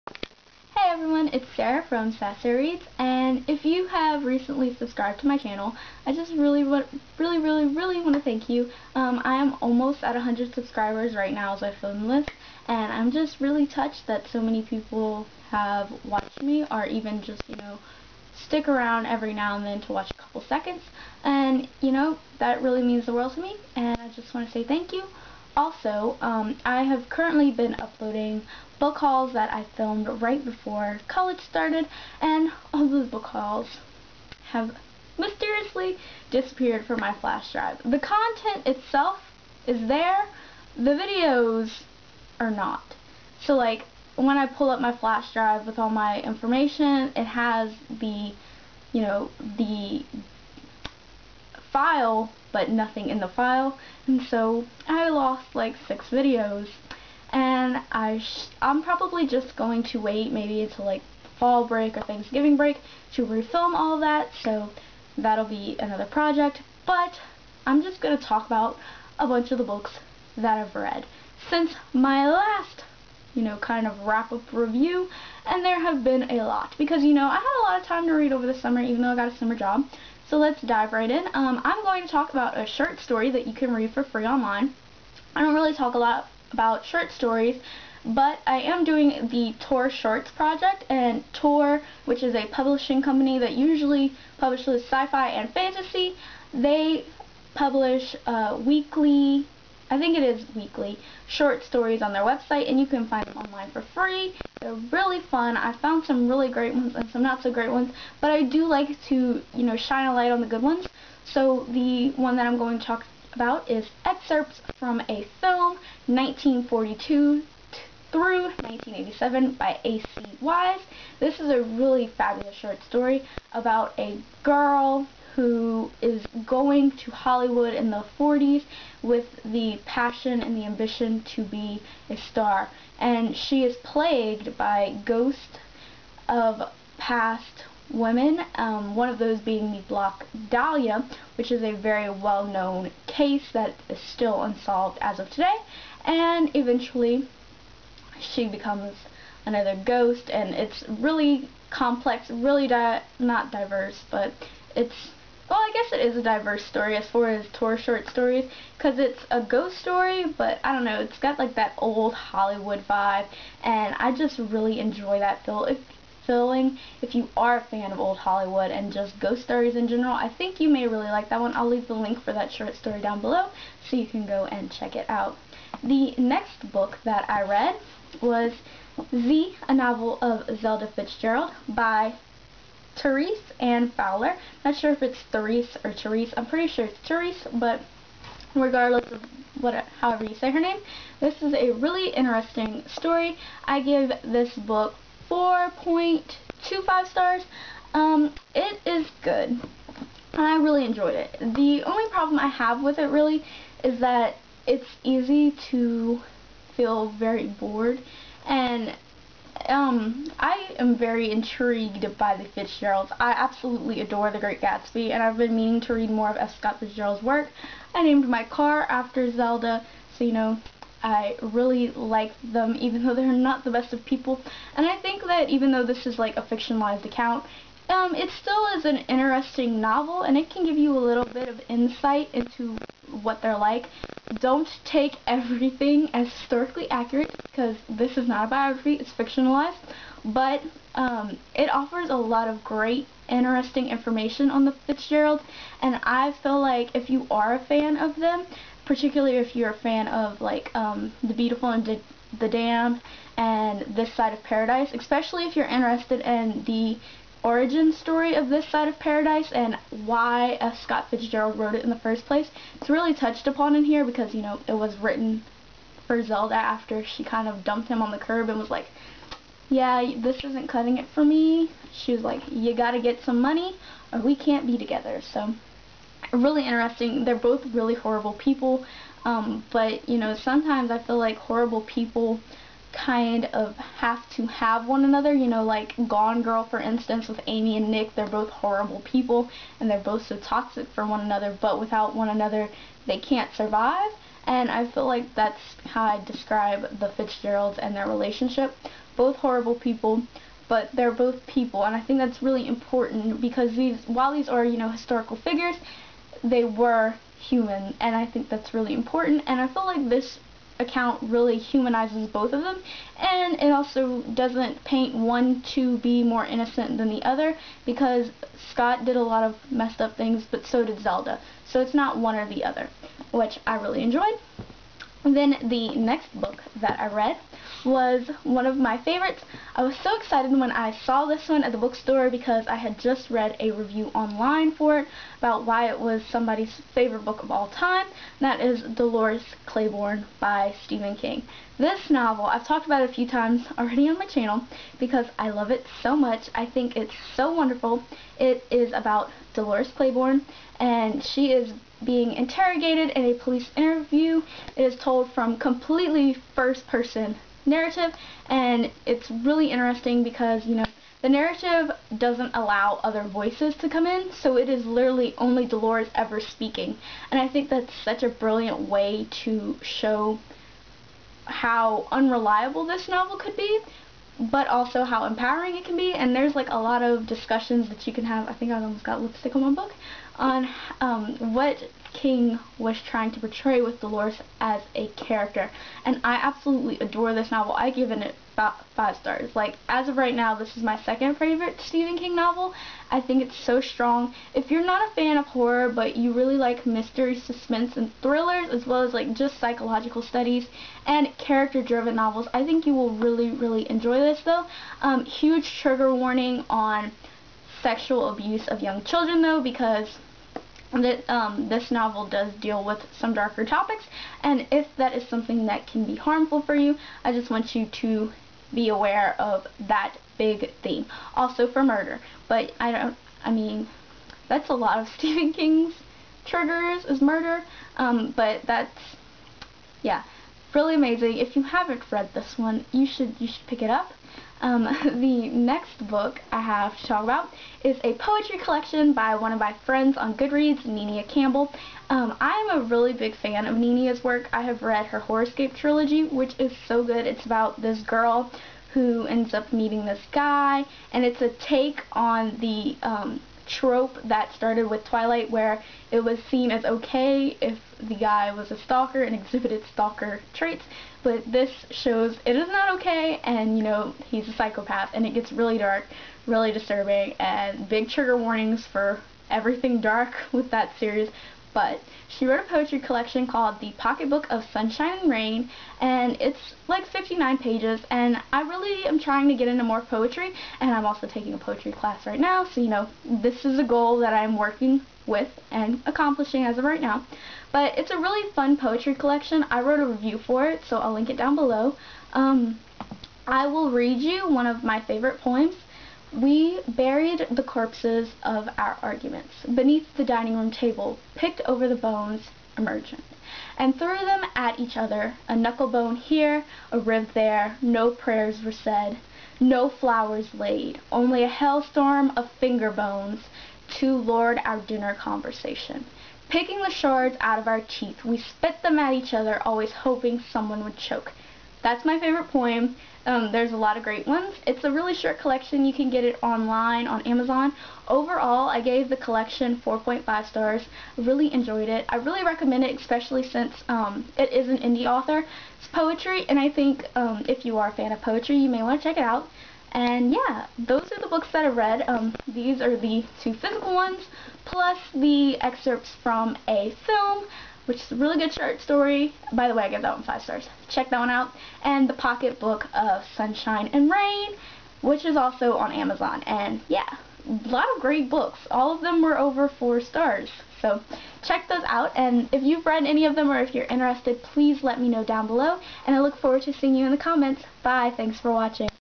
Books I've Read in 2017 #20-23 + A Big Thanks
I'm currently over 100 subscribers and it means the world to me. Much love and happy reading!

~Have you read any of these books? What are your thoughts on them? What are some books you'd recommend? Let me know down below in the comments.